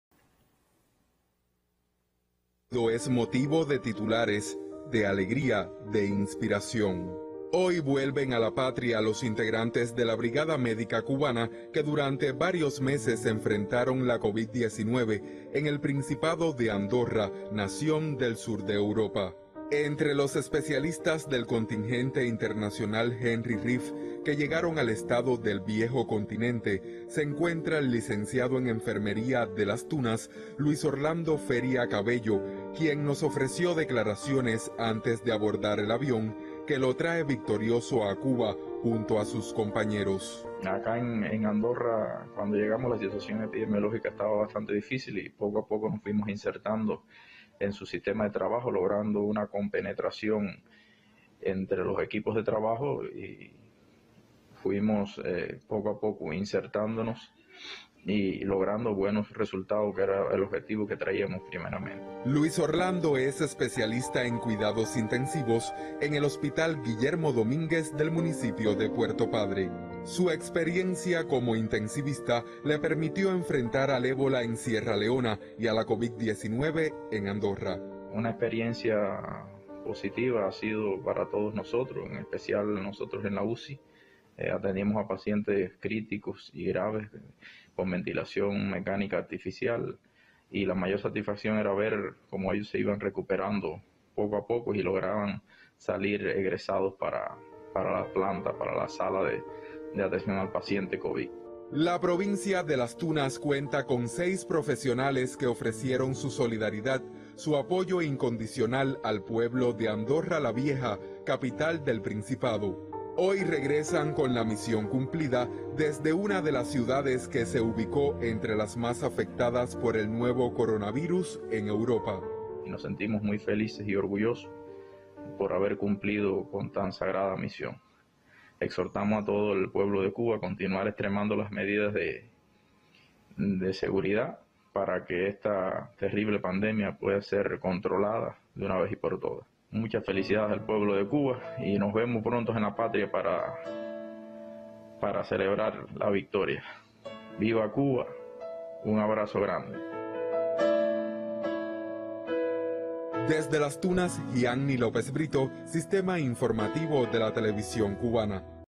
Cuba: Profesionales de la salud de Las Tunas laboraron en Andorra contra la pandemia
Varios son los profesionales de la salud de la provincia de LasTunas, integrantes de la BrigadaMédica cubana que regresa hoy de Andorra. Con uno de ellos conversó vía Internet nuestro corresponsal Gianny López Brito.